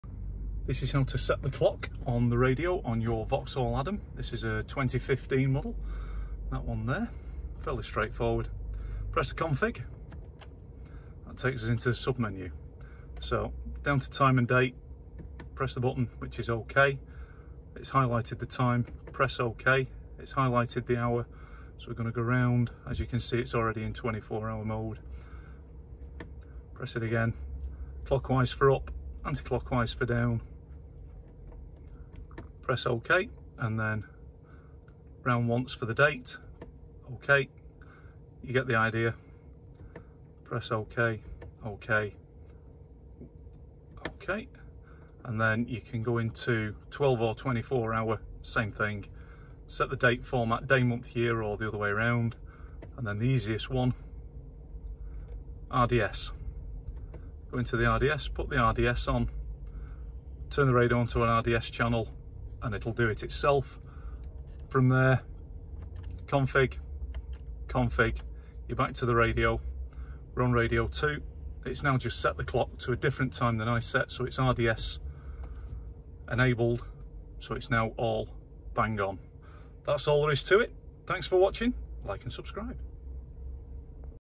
Vauxhall Adam Clock Setting How to set the Time Reset the Clock
This is how to set the clock on a Vauxhall Adam